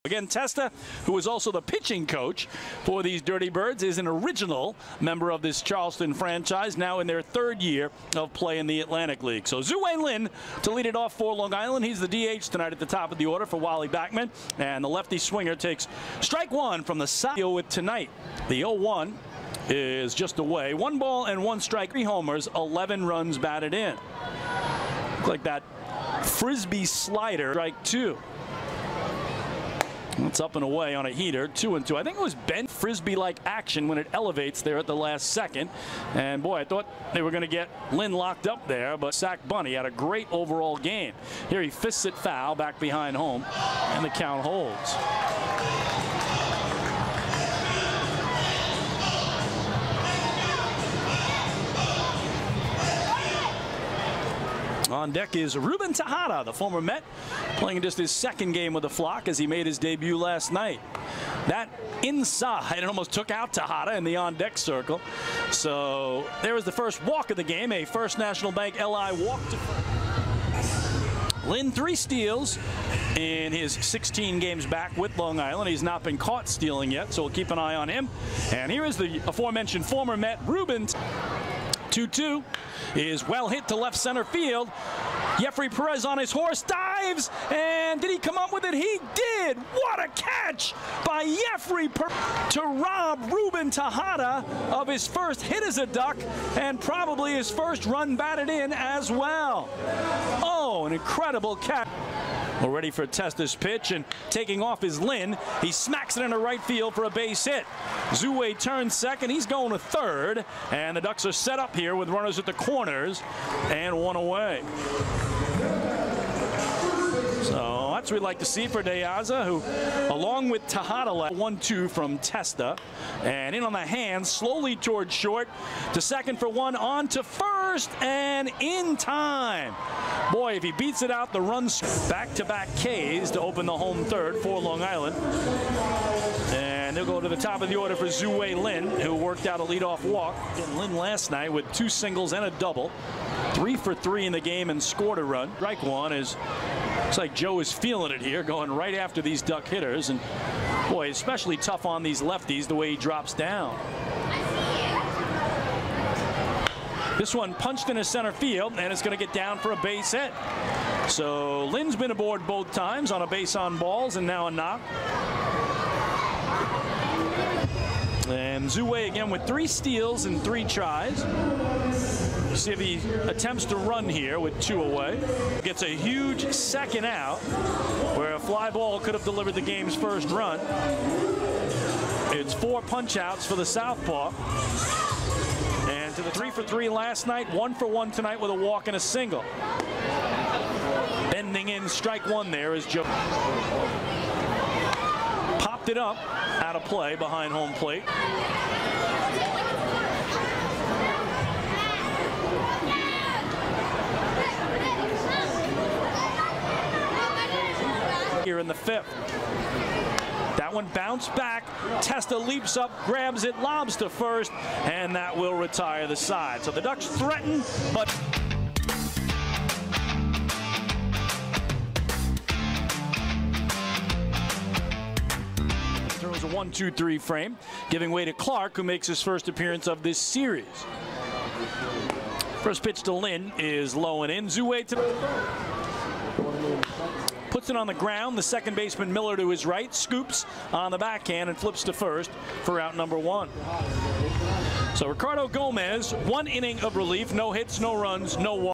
20230615林子偉(長島鴨)DH全場｜怪姿投手照敲安！安打加保送,稱職開路先鋒！繼續維持打擊好手感！
20230615林子偉[獨聯-長島鴨]DH｜1-3,1B,BB
大西洋職業棒球聯盟
累積打擊三圍
[346/404/635  ops 1.039]